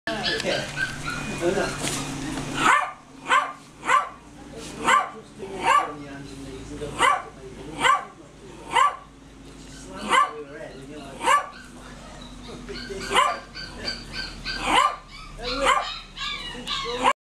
Buddy
Ik ben net geschoren, maar nu heb ik een roze strikje in mijn haar! Toch ben ik een mannetje hoor!